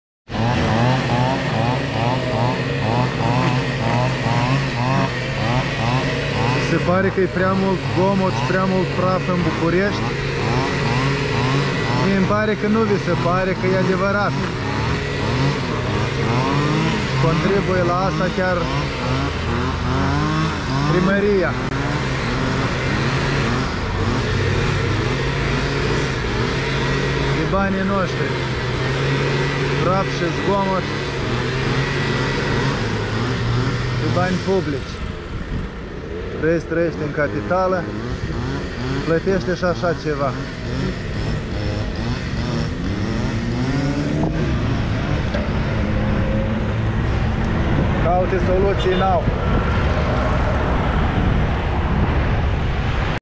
Serviciile municipale care poluează cu zgomot și praf - Curaj.TV
Oamenii ăștia tund iarba cu o coasă zgomotoasă, apoi o suflă în toate părțile stîrnind nori de praf cu un alt aparat motorizat. Vă mai mirați că e poluată capitala în ultimul hal?